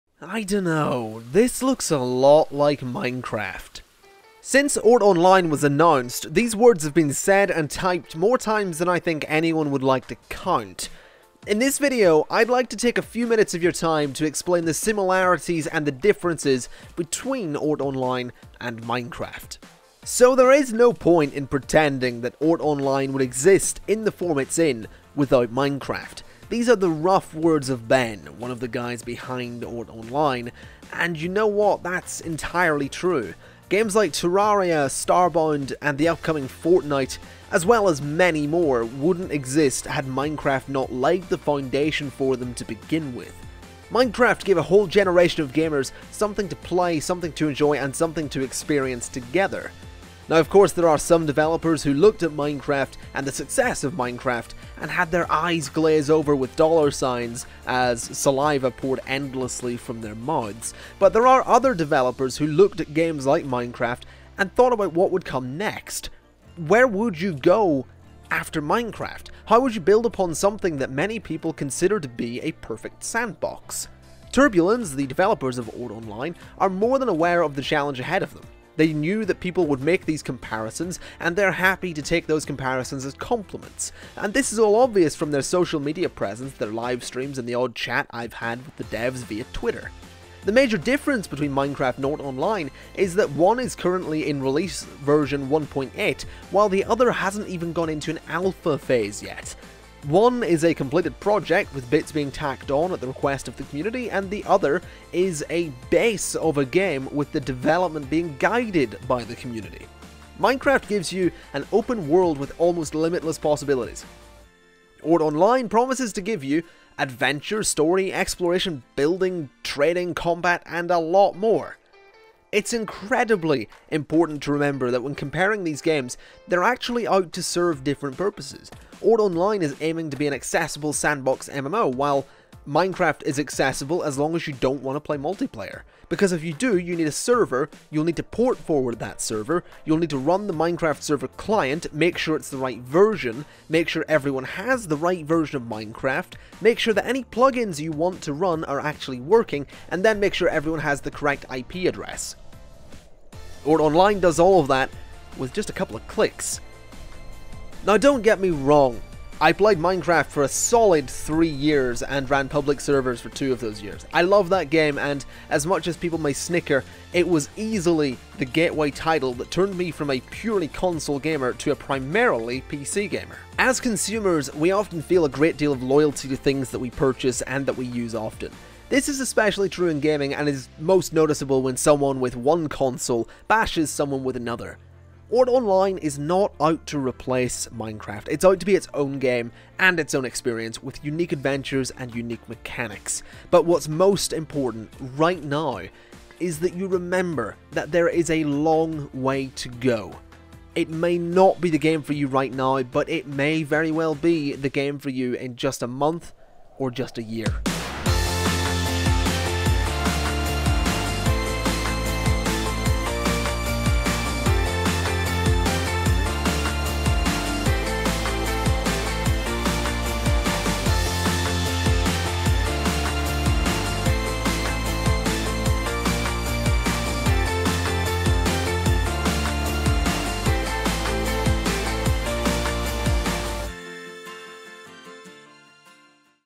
THE MINECRAFT DISCUSSION | Oort Online
A fair few people write Oort Online off because it "looks like Minecraft" - here's my two cents...

For the best prices on all of the latest games, check out cdkeys.com!

► TwitchTV...

► TwitchTV...

► PC Specs...